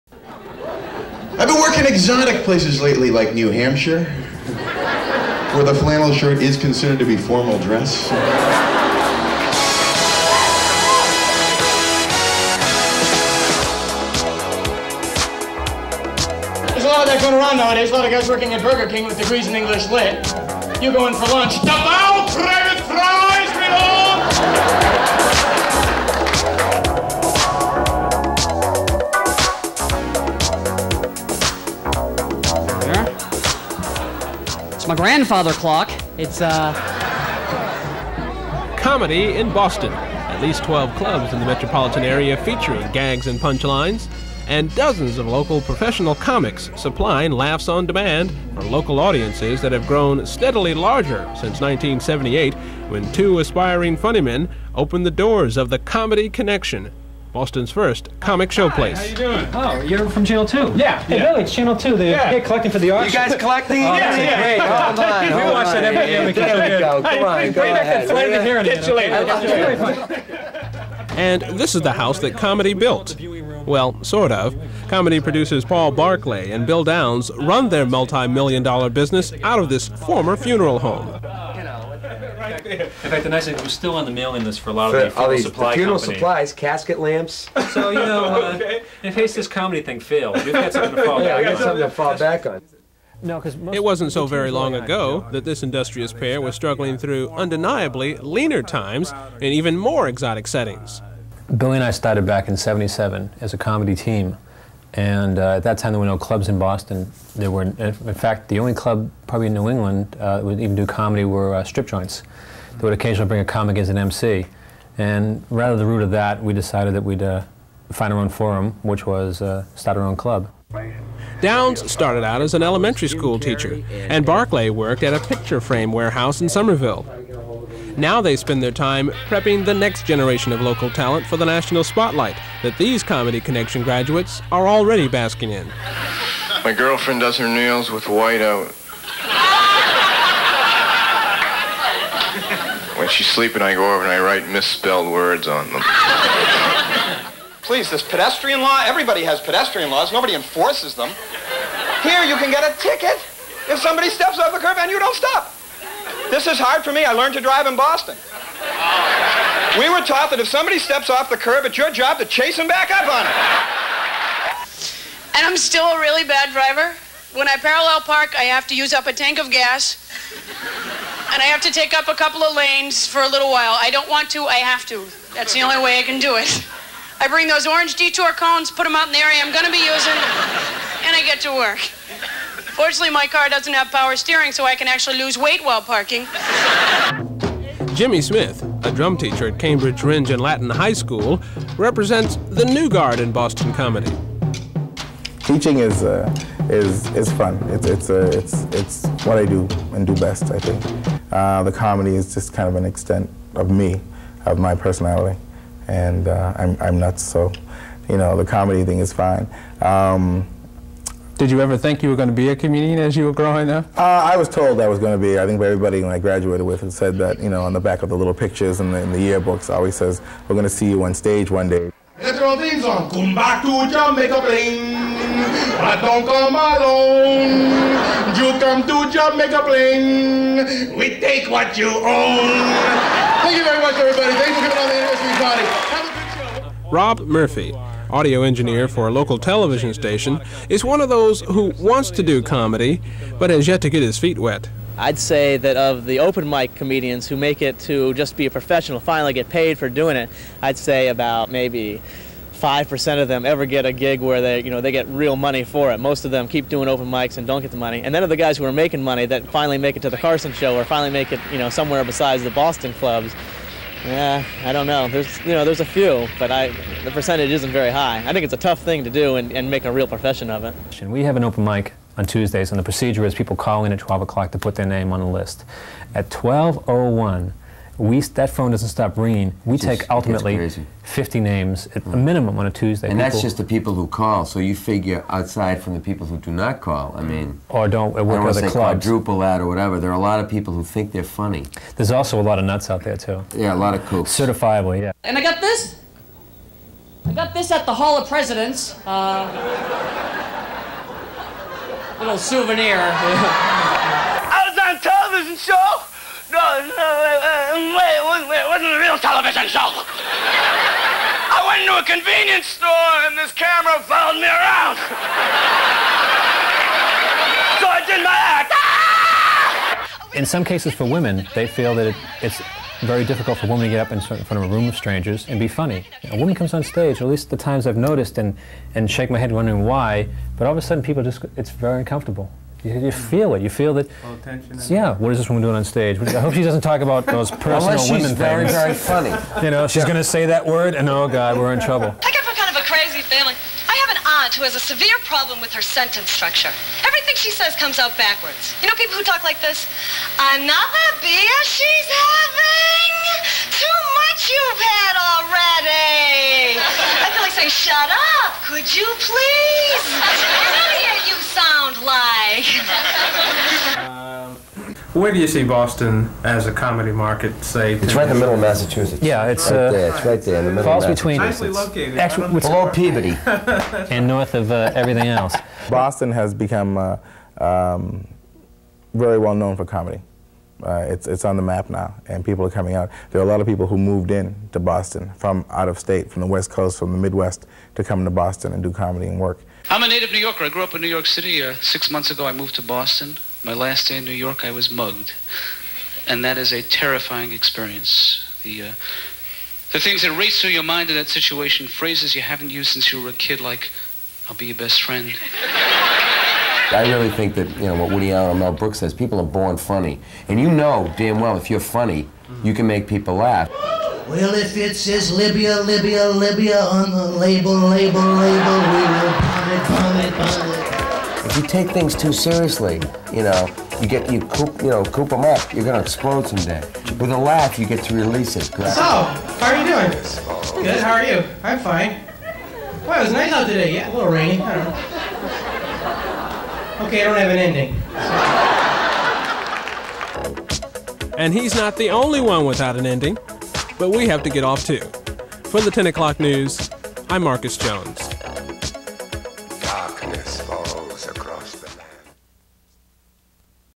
The COMEDY CONNECTION - How Boston Became a Hotspot for New Talent
From the time capsule:  Jay Leno, Steven Wright, Denis Leary, Lenny Clarke, Dane Cook and Louis C.K. - just a few of the nationally-known comedic talents who cut their professional teeth in Boston.  During the 1980s, live comedy became all the rage in places such as New York, Los Angeles, Chicago and San Francisco.  And then, as Marcus Jones reports, Boston got into to act fielding its own crop of players.